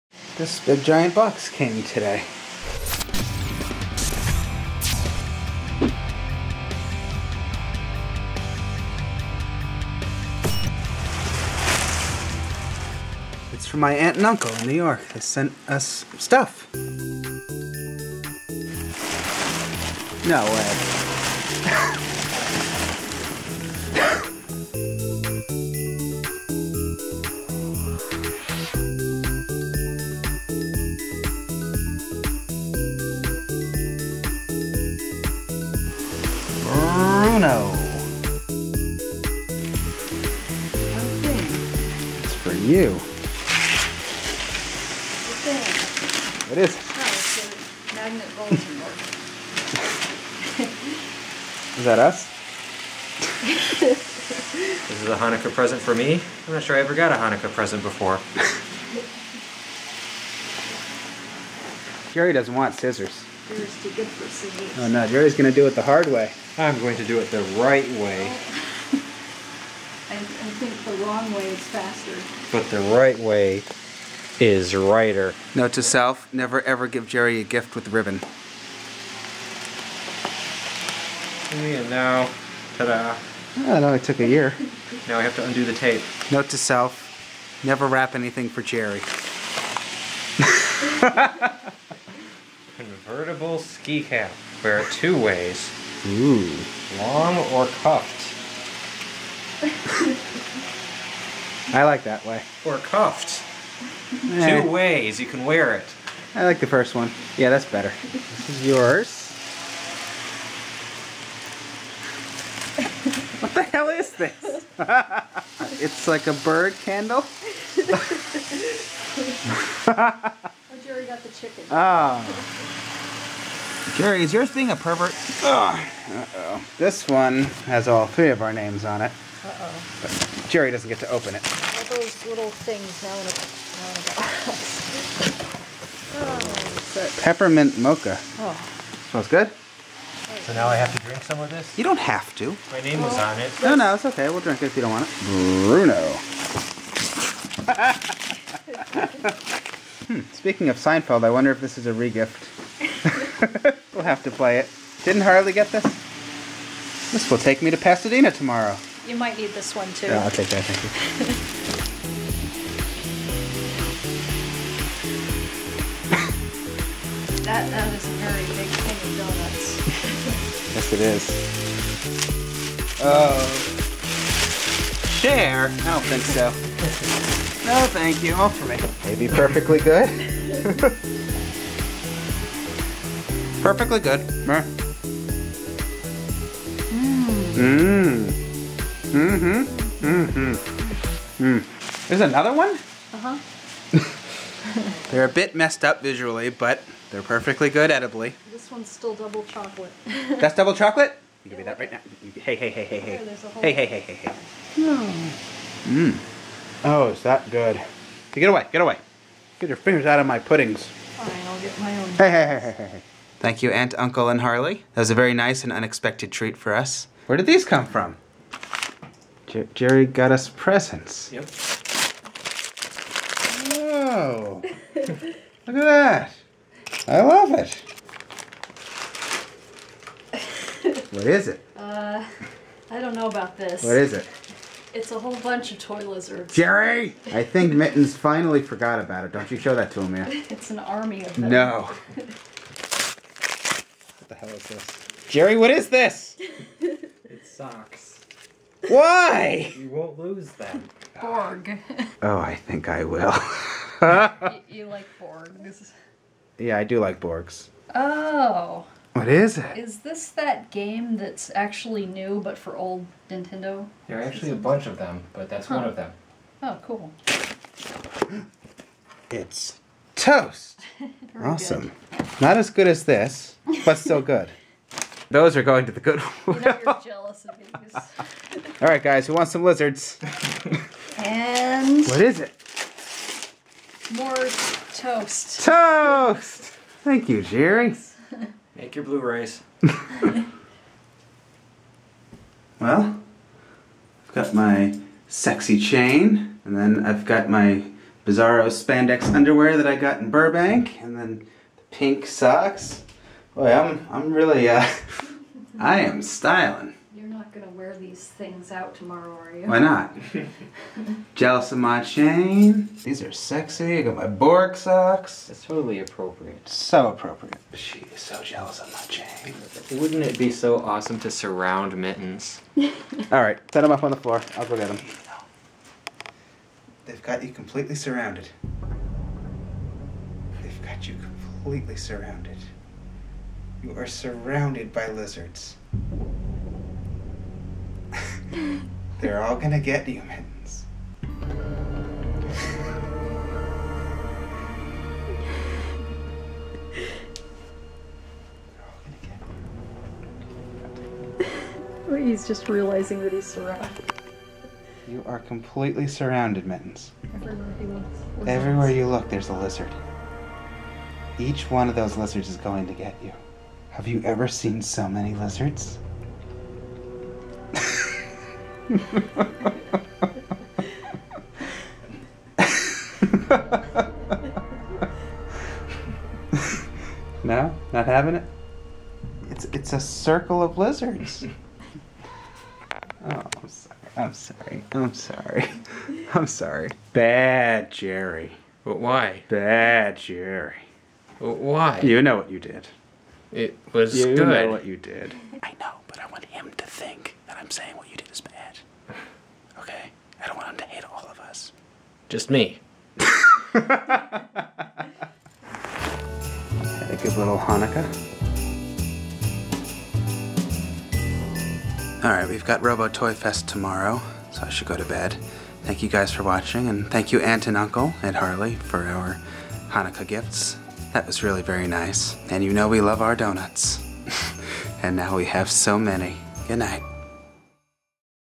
Hanukkah Presents! (12.15.12 Vlog 244)
My Aunt & Uncle sent us a box of goodies from NY! (Thanks guys!)

►CAMERA “GOOD LENS” Sony Mirrorless Camera
►CAMERA Canon SX740: (Small VLOG Camera)
►CAMERA GoPro Hero11